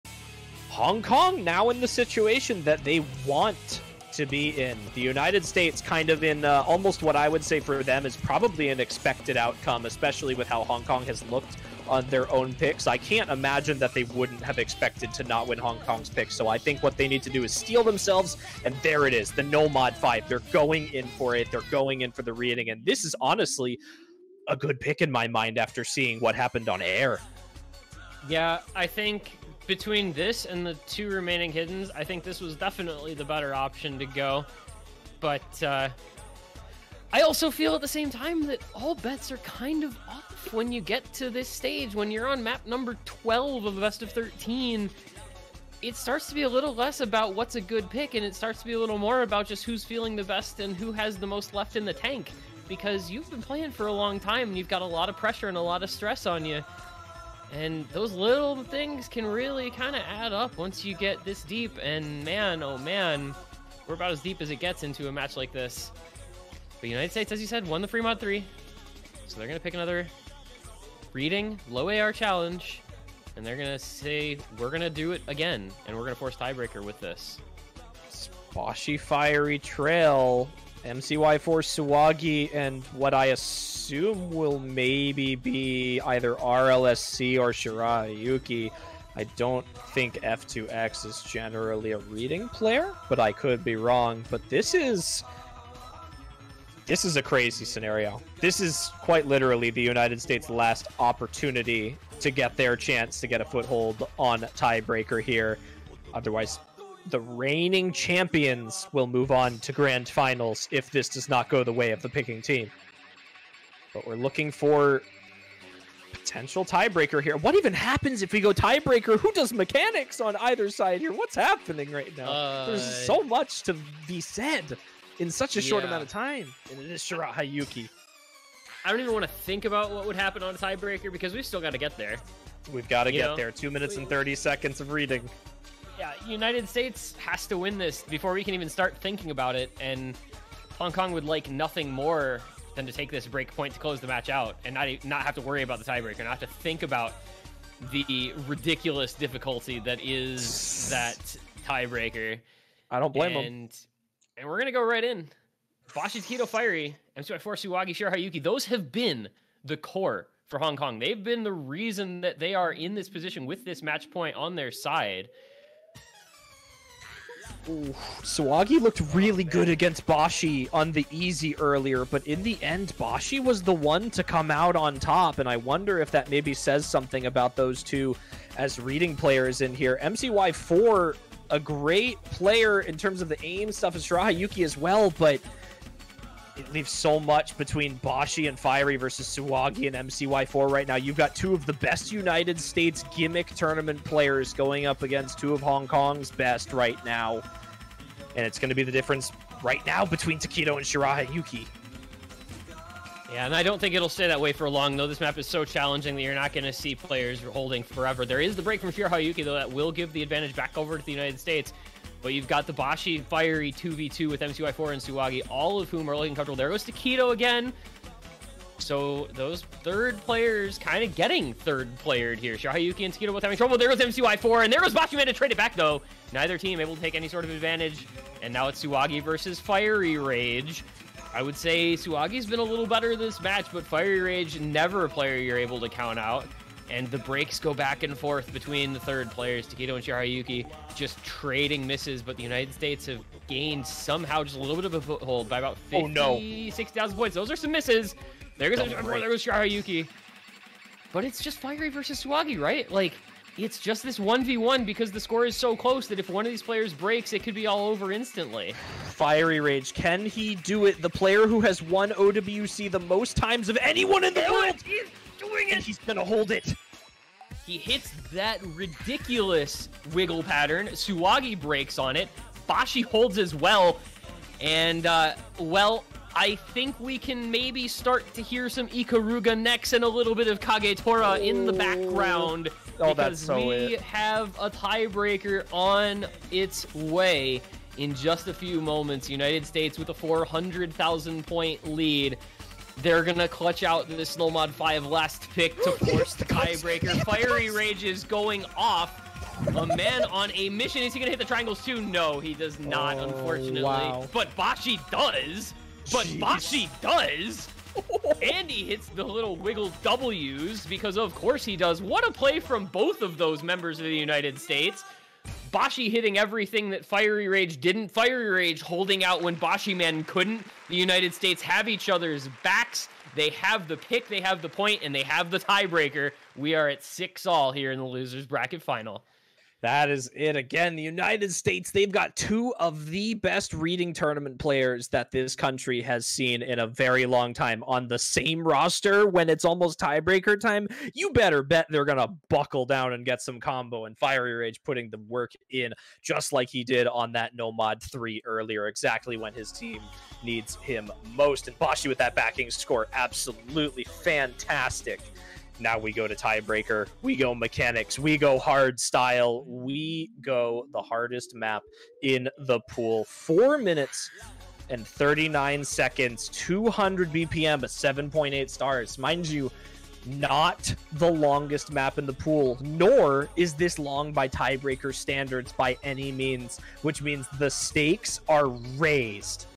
LAB=01 3xMiss during 3wc match
This map was NM5 during 3WC grand finals and we picked it as our last pick into Hong Kong to go to the tiebreaker. Unfortunately, we did not win the tiebreaker and we exited the tournament with a third-place finish, but I still think everyone on the USA team performed exceptionally well and I wanted to show this cool performance by me and fiery that happened. For reference by the way, my 2nd best nomod score on this map was a 336k 14xMiss so this scores maybe a just a biiiiiiiiiiiiiiiiiit above what I thought I would get.